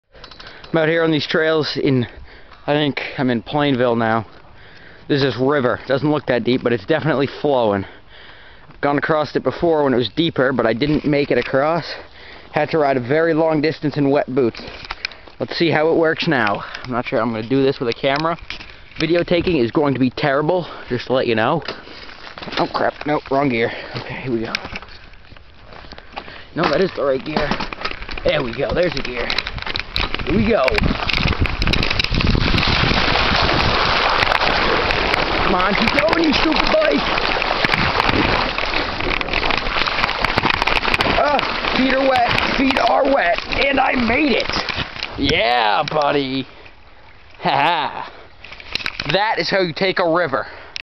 river crosing on a bike
this trail is well used but has a few river crossings this is the only one i would hold my camra whil trying